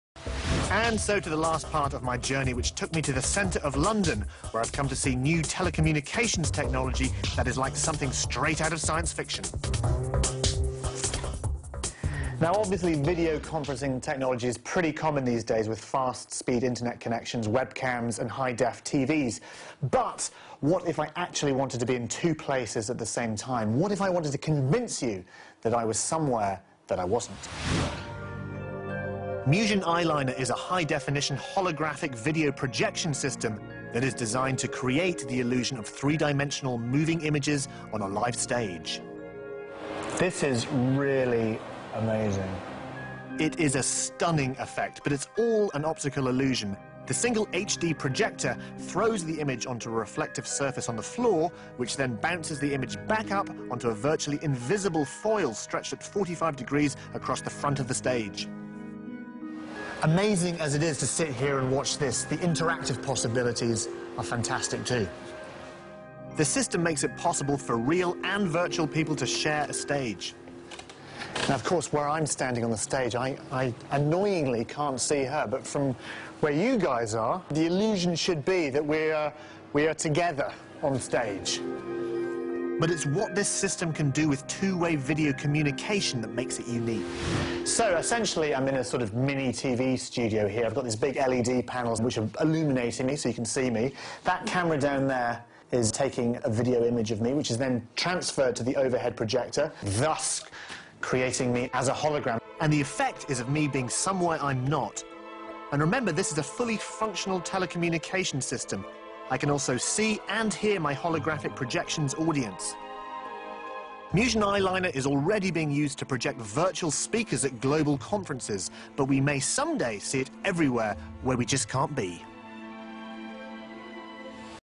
How coachella made the 2pac hologram appear
A short explanation of how the technology works that brought 2pac back to life! Super interesting!